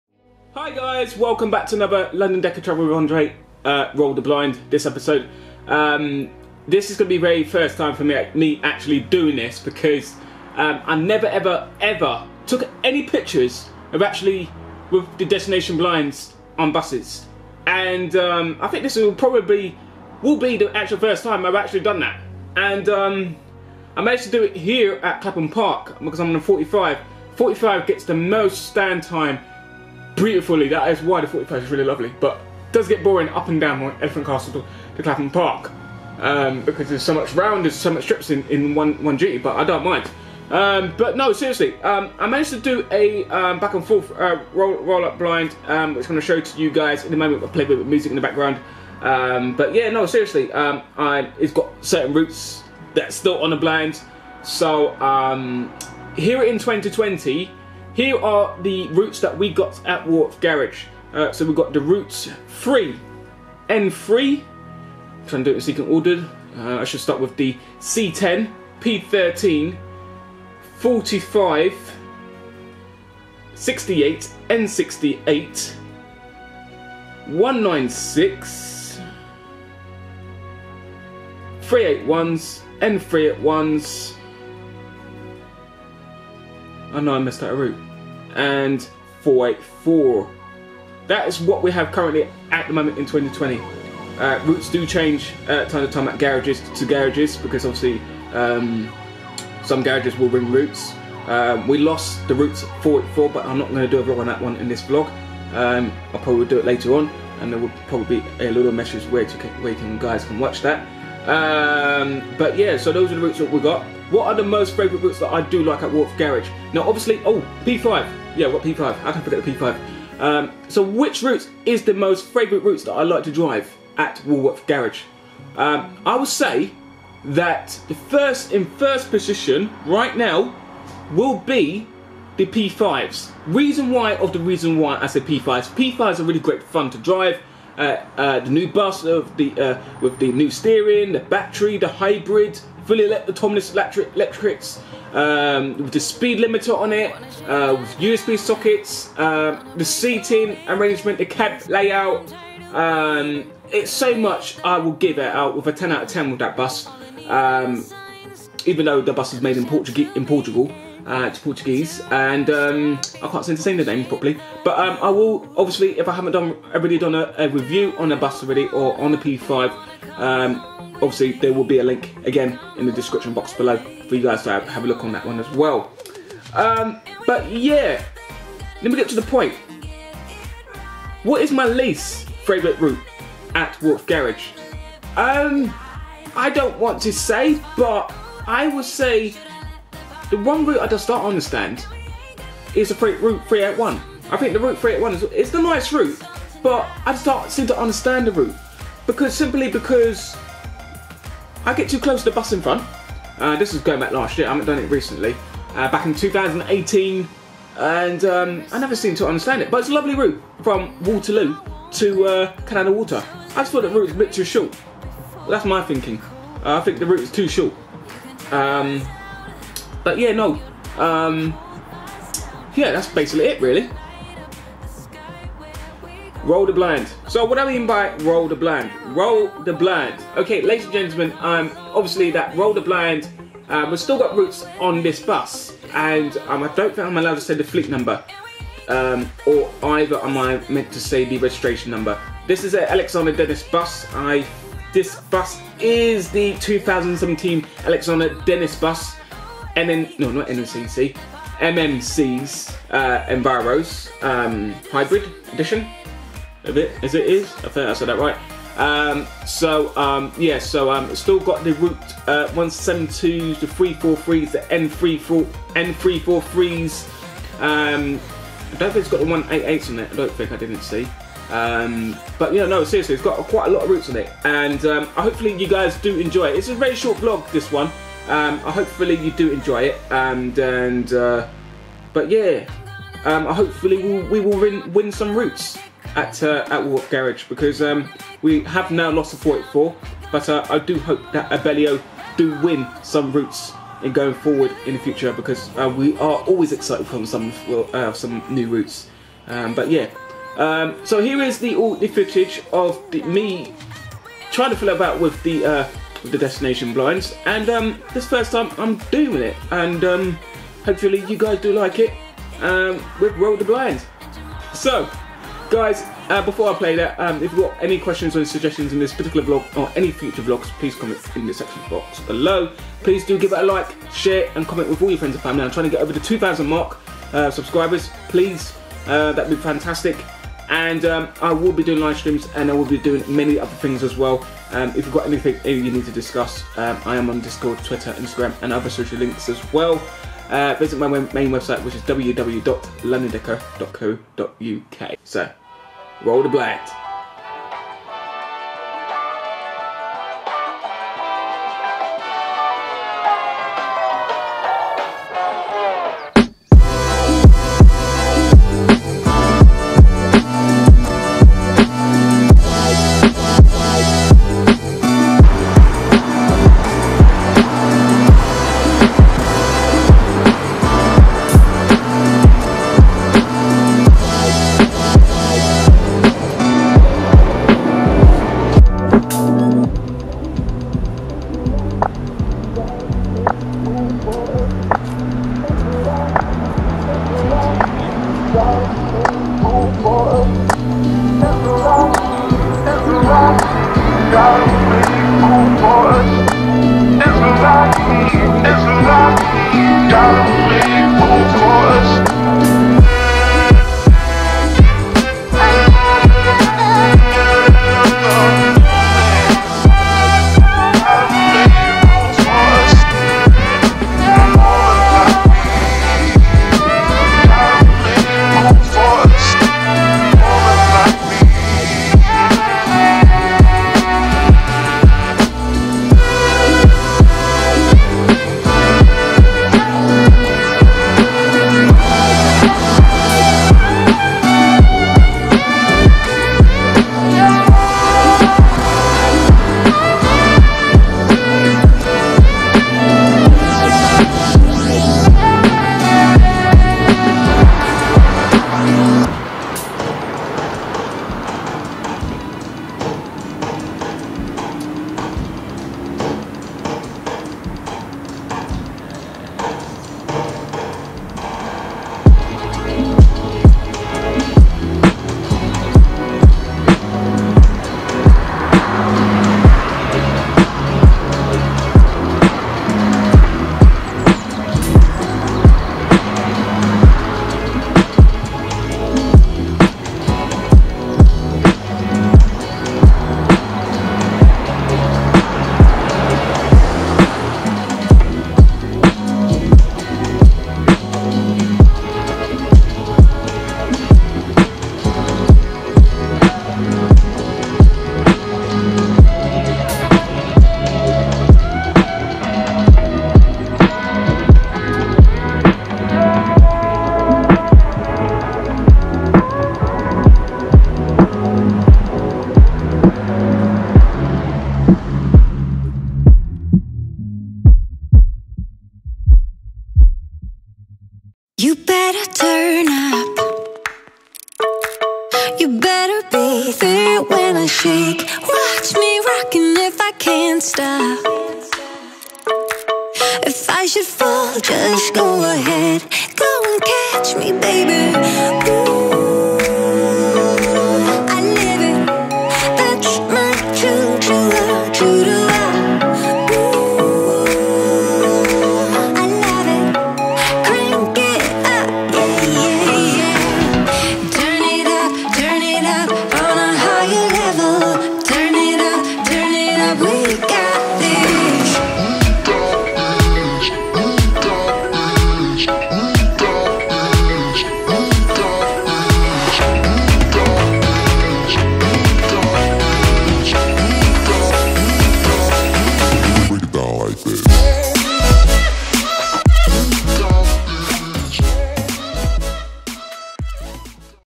ROLL THE BLINDS
Hello Everyone in today vlog I am excited to talk about: Roll The Blind on a Alexander Dennis MMC Enviro 400

**I MUST REMIND EVERYONE, please don't use negative words or any insulting vocabulary on any of London Decker social media site **(PLEASE BE FRIENDLY)**